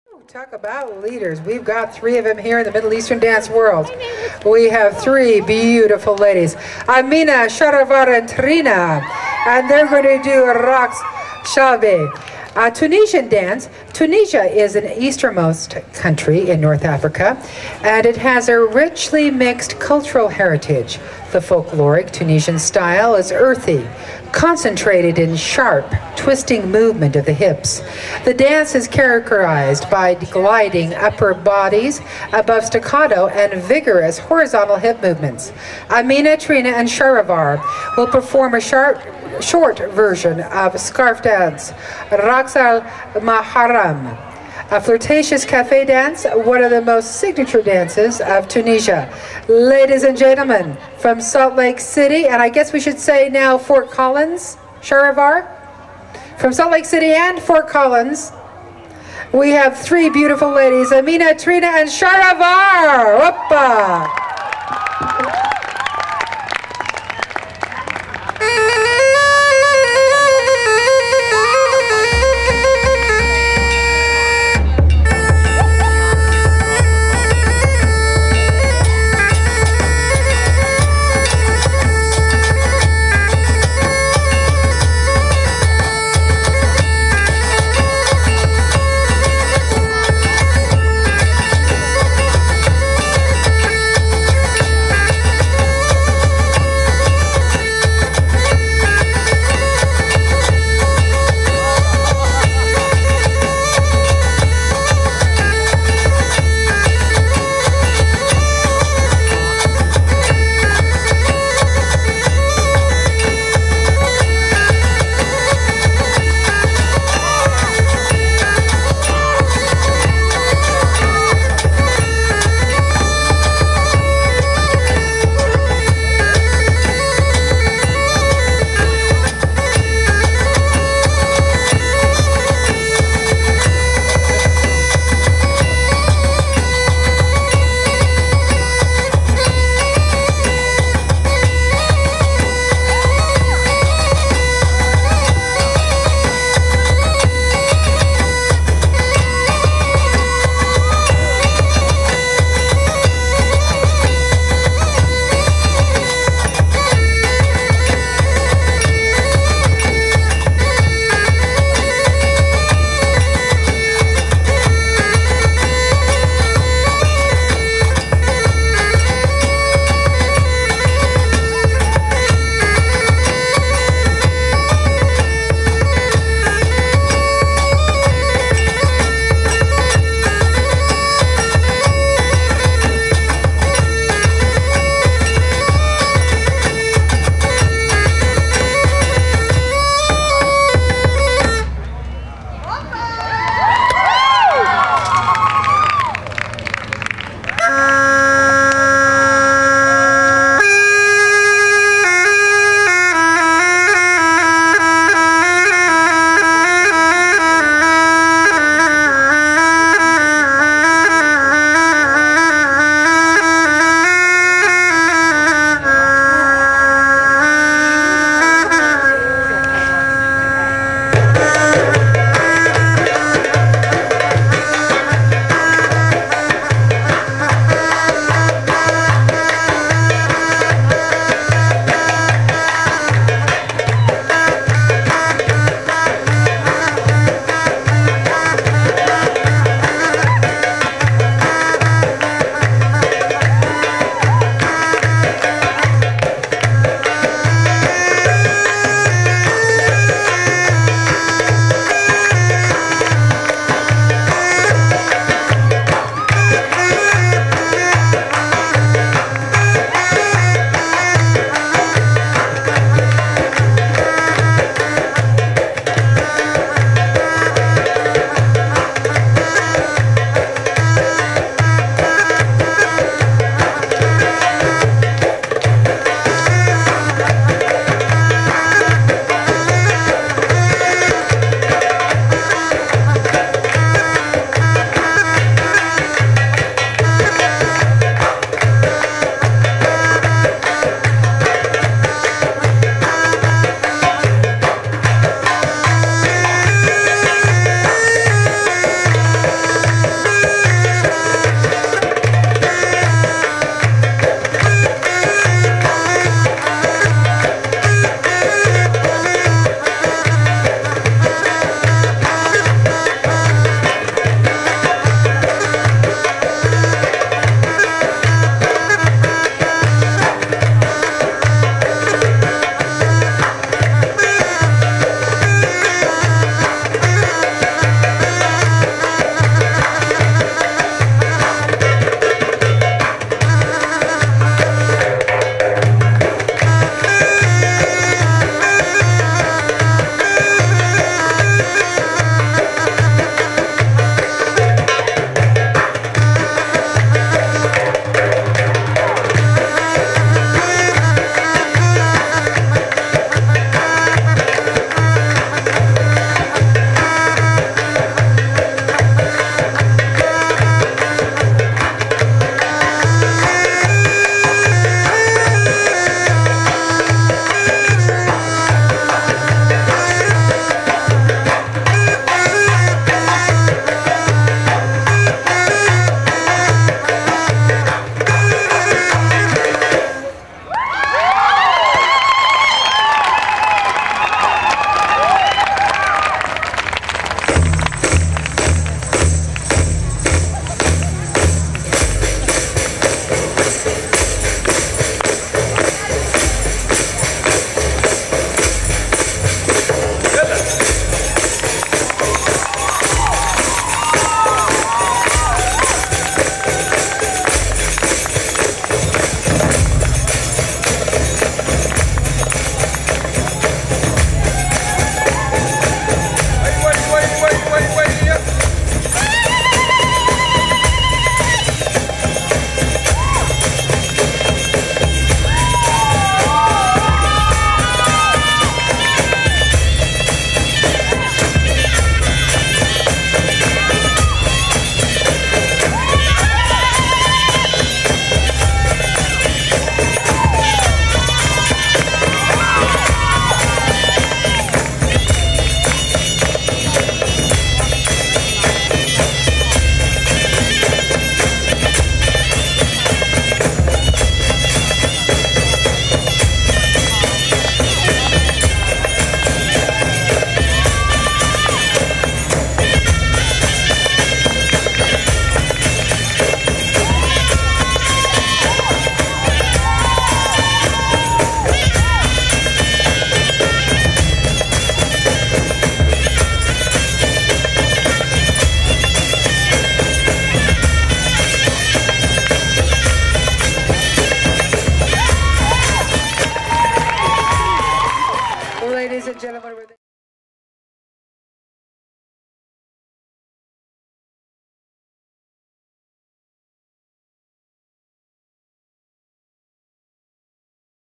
Amina Trina Shahravar Tunisian Trio!
This fun Tunisian style dance has many of the Traditional characteristics of Authentic Tunisian Folk dances.
This is an entertaining piece.
Tough to film.
The stage is about 5 feet off the ground so you see all of our steps looking up a bit more than oue would hope. Many of the accents are lost from this angle.
Slc, Utah 26th Annual Belly Dance Festival.
August 2006.
Choreography & Costumes By; Amina!